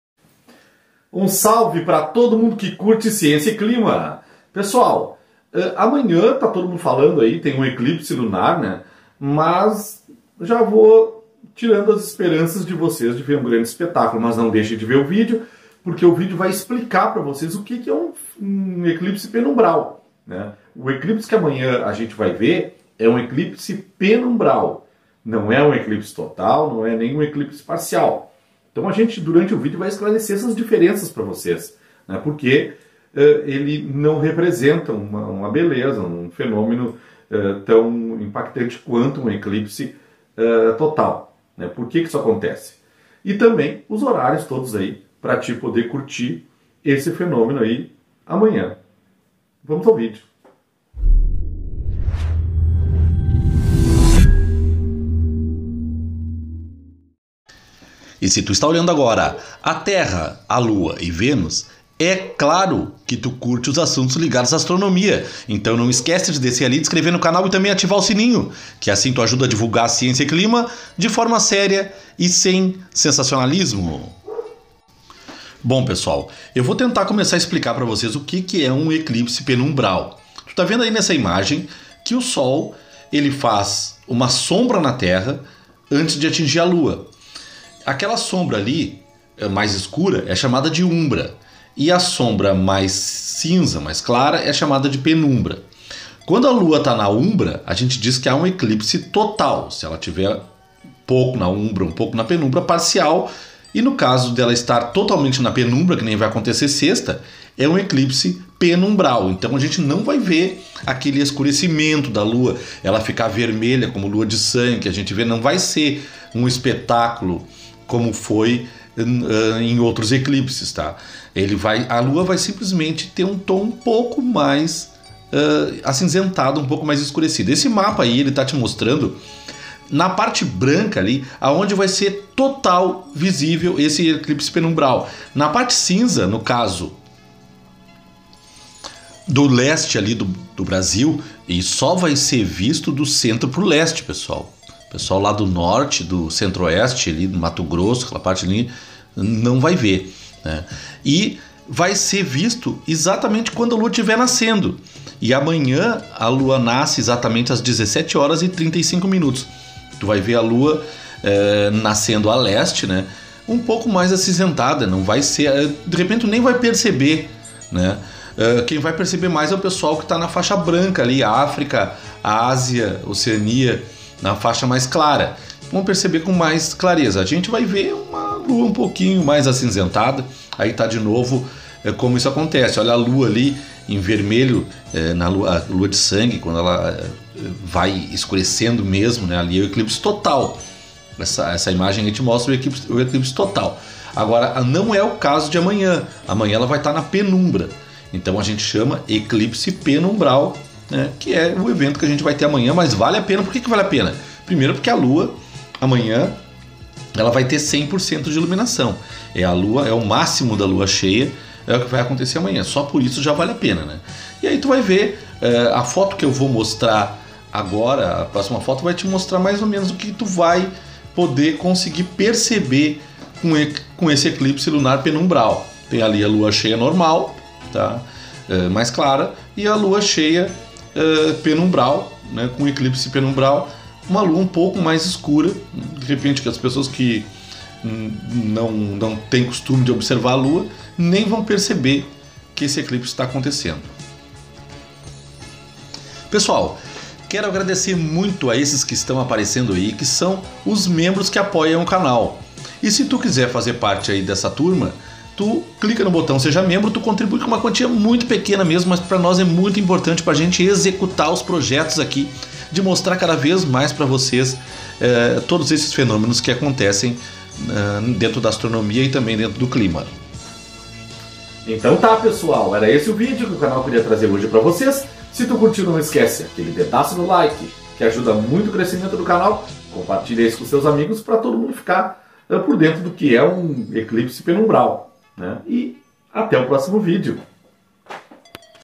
Amanhã 05/06 - Eclipse Penumbral da Lua
este tipo de eclipse ocorre “quando a Lua entra na região da penumbra da Terra” e esclarece que pode ser identificado através de uma “variação do brilho da Lua”. Porém, é notado que este brilho também não é notado com facilidade.

O Eclipse Penumbral poderá ser visto no nascer da lua, as 17:35h desta sexta-feira